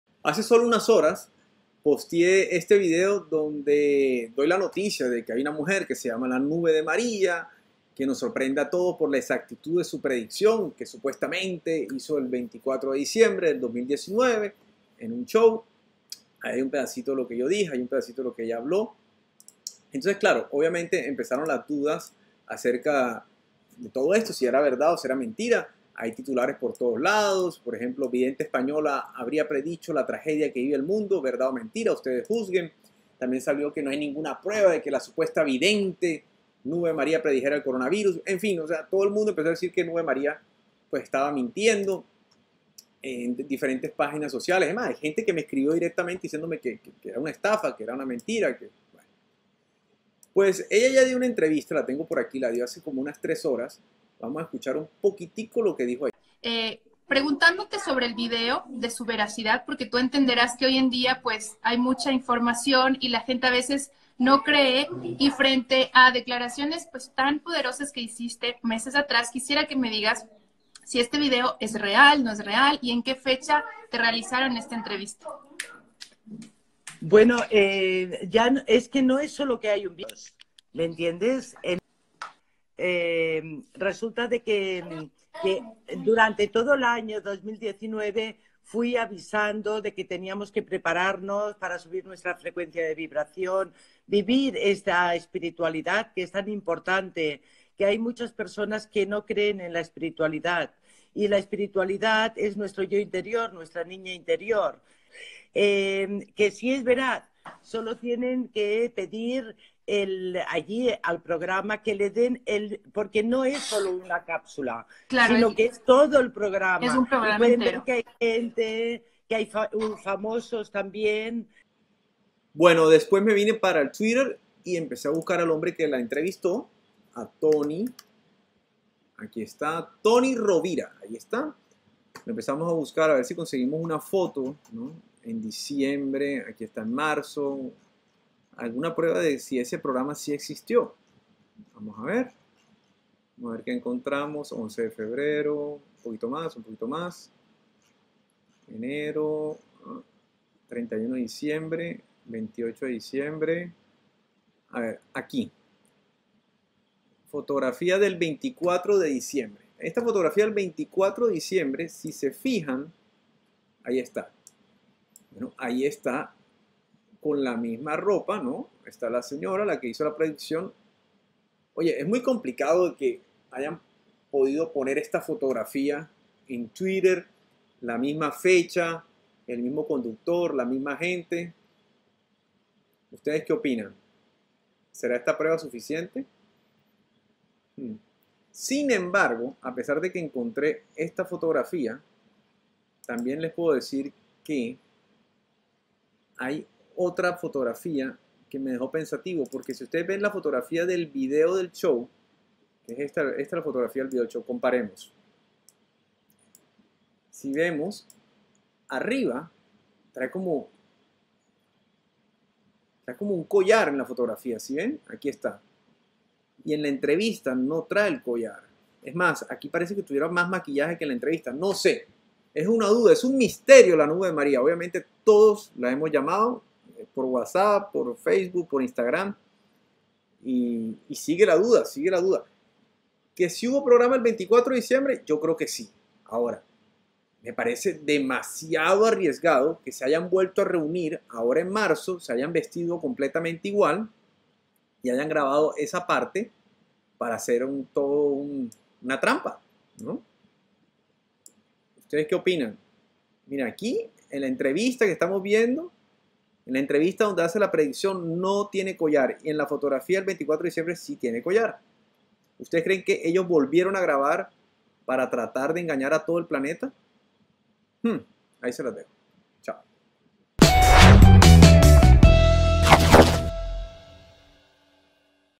¿ES UN MONATAJE DE ' LA NUBE DE MARIA' SU PREDICCION DEL CORONA VIRUS?
MAS DATOS, FOTOS Y VIDEOS PARA QUE USTED DECIDA SI PREDIJO O NO ESTA MUJER EL CORONA VIRUS